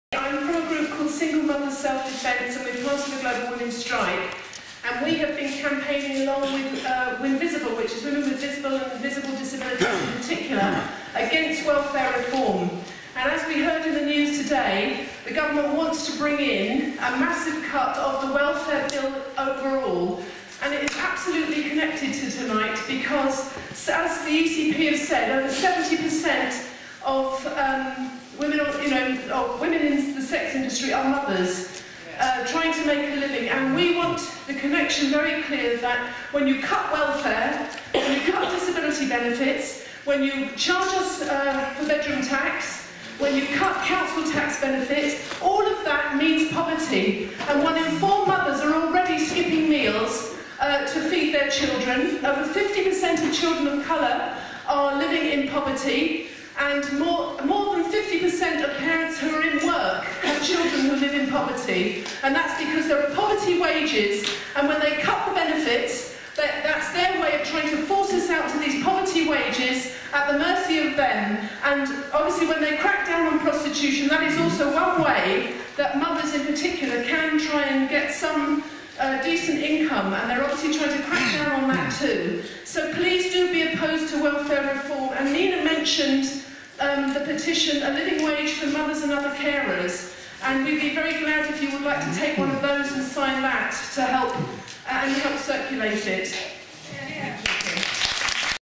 Kim Sparrow Single Mothers' Self Defence v Criminalisation of Sex Work
PEOPLE'S PARLIAMENT MEETING, HOUSE OF COMMONS.  Stop the criminalisation of sex work -- safety first!  26 March 2014